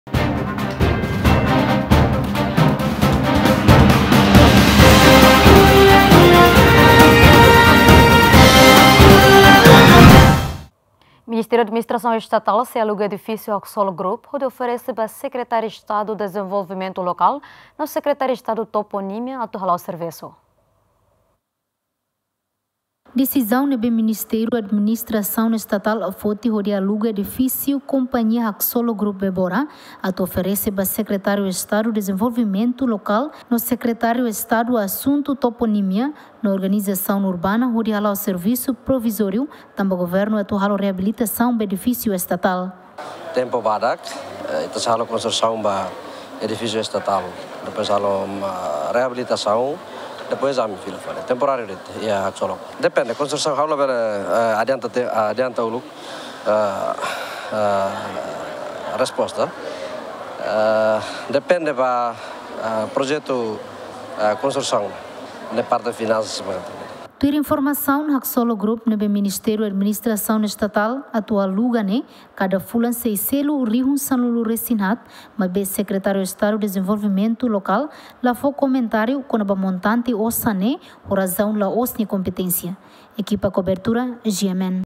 MAE sei aluga edifísiu Haksolok Group hodi oferese ba SEDL no SET atu hala'o servisu
MAE sei aluga edifísiu Haksolok Group hodi oferese ba Sekretária Estadu Dezenvolvimentu Lokal no Sekretaria Estadu Toponímia atu hala'o servisu